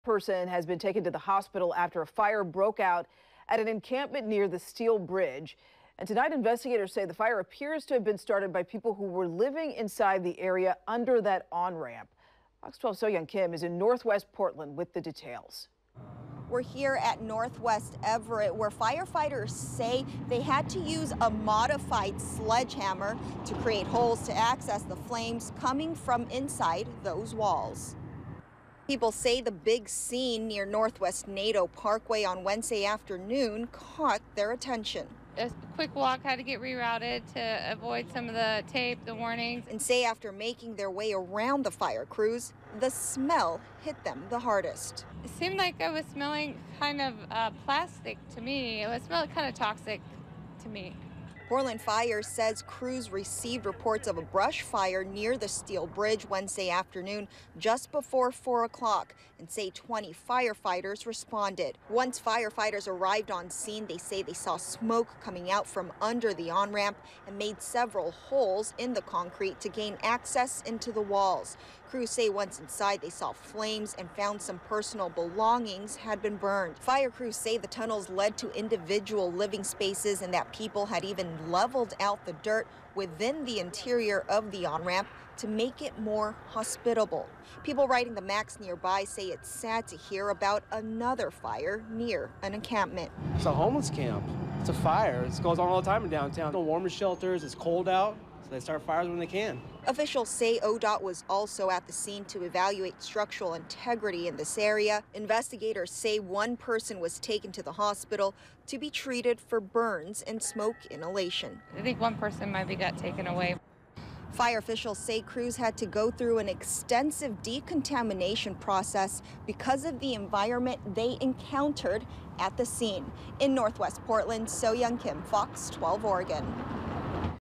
Fire reveals homeless camp inside Portland bridge on-ramp
One person has been taken to the hospital after a fire broke out at an encampment inside a hollowed-out portion of the Steel Bridge on-ramp.